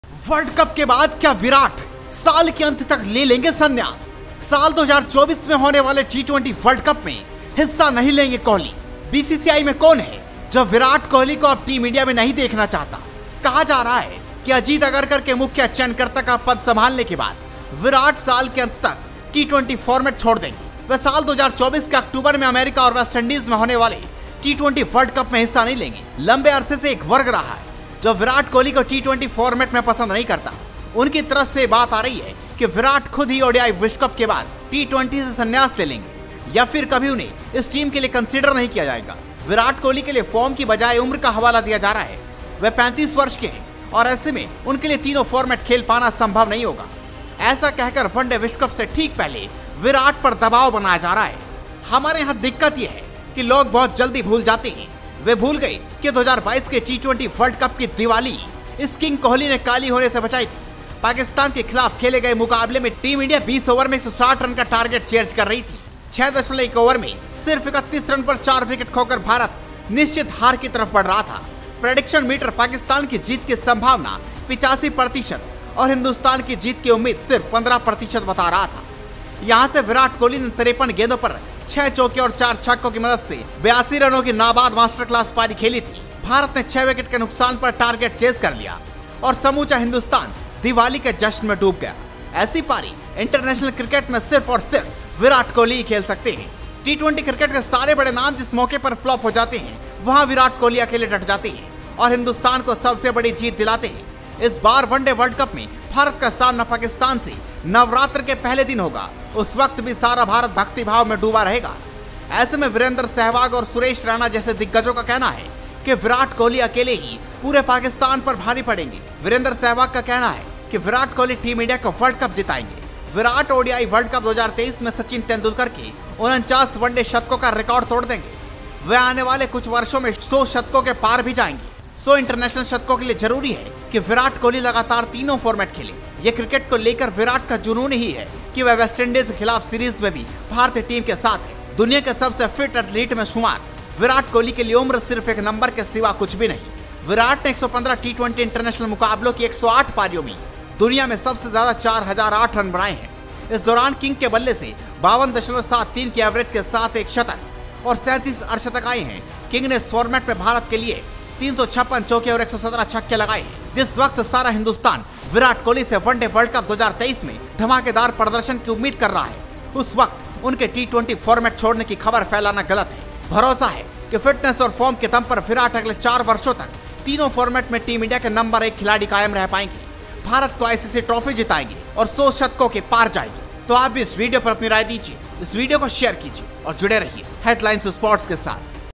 दिवाली की रात पाक से जीत दिलाने वाले विराट पर कौन बना रहा संन्यास का दबाव?| Headlines Sports
दिवाली की रात पाक से जीत दिलाने वाले विराट पर कौन बना रहा संन्यास का दबाव?